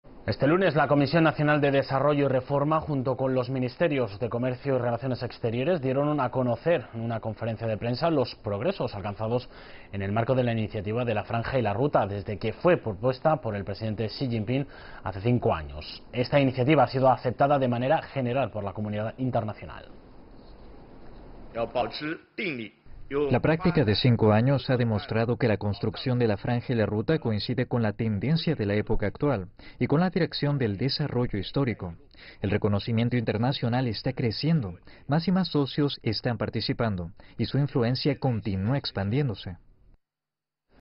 Progresos alcanzados en el marco de la Iniciativa de la Franja y la Ruta de los últimos cinco años
Este lunes, la Comisión Nacional de Desarrollo y Reforma, junto con los ministerios de Comercio y Relaciones Exteriores, dieron a conocer en una conferencia de prensa los progresos alcanzados en el marco de la Iniciativa de la Franja y la Ruta, desde que fue propuesta por el presidente Xi Jinping hace cinco años. Esta iniciativa ha sido aceptada de manera general por la comunidad internacional.